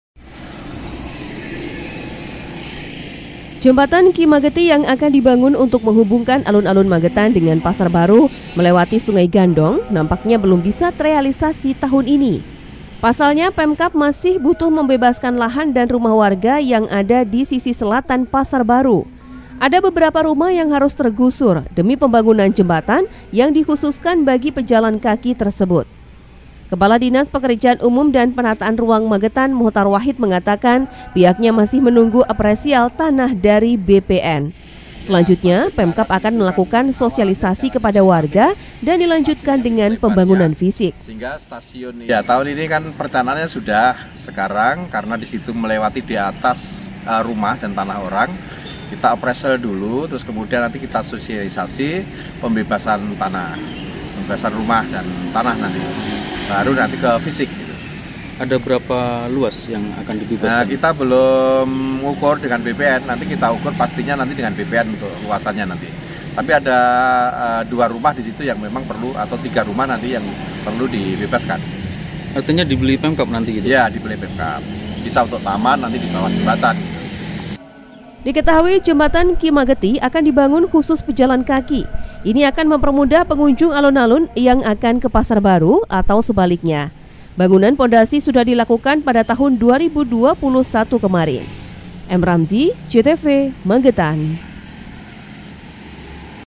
MAGETAN - Bebaskan Lahan Warga, Untuk Bangun Jembatan Ki Mageti
Pemkab Magetan masih butuh membebaskan lahan atau rumah warga untuk melanjutkan pembangunan jembatan Ki Mageti yang rencananya diperuntukkan khusus pejalan kaki. Pemkab masih menunggu appraisal tanah dan bangunan.